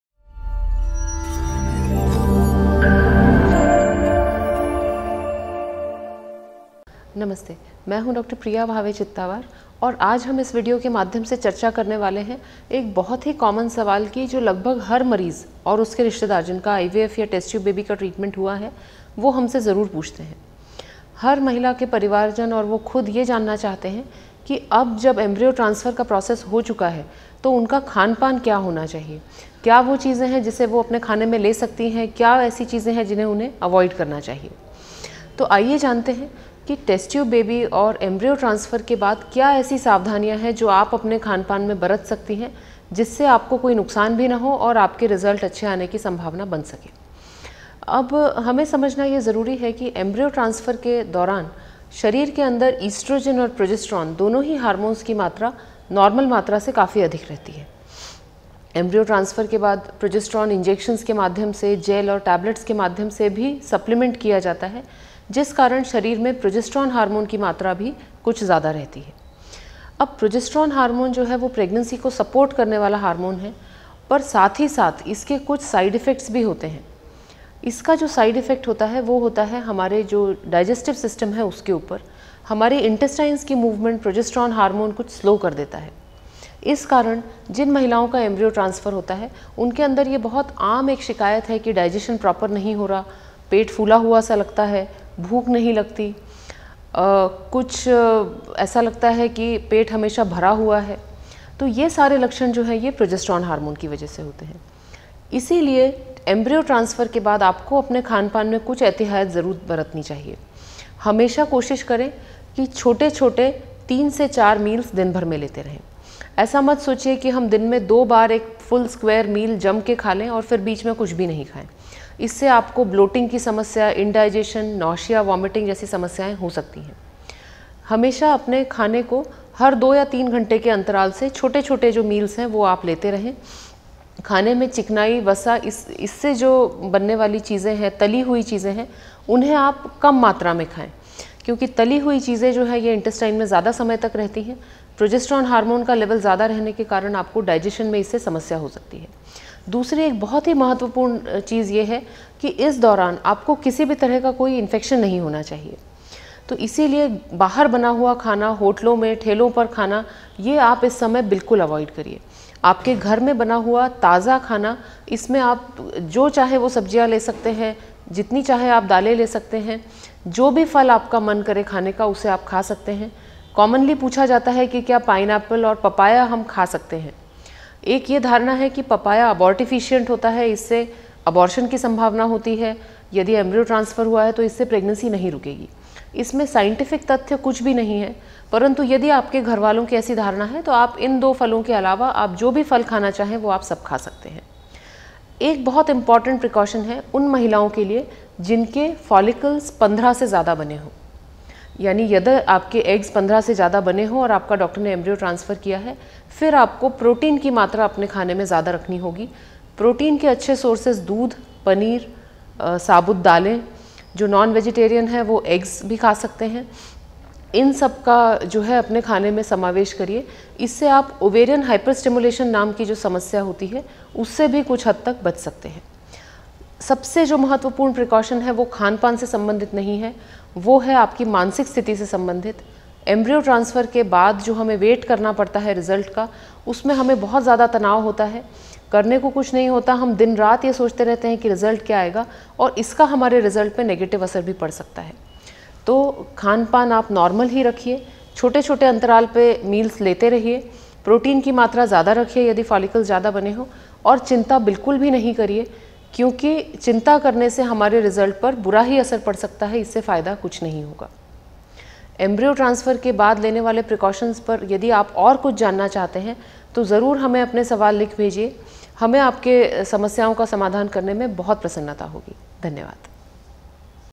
What to eat after IVF? ivf के बाद आहार..
Learn what to eat after IVF cycle from Dr. Priya Bhave Chittawar